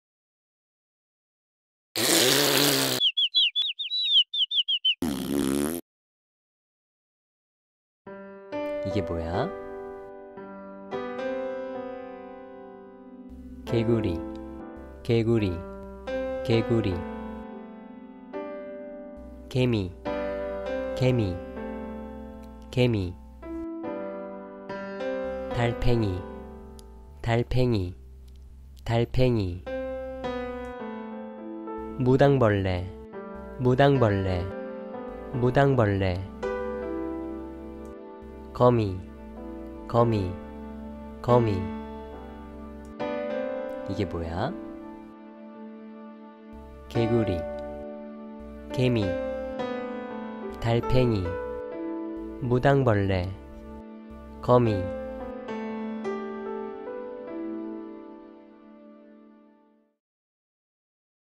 말배우기 _ 곤충 이름 외우기 _ 곤충 _ 2화 _ learn Korean
곤충들을 보며 이름을 말해 보아요
이게 모야? 라고 묻는 아이들에게 곤충들의 이름을 알려주세요~^^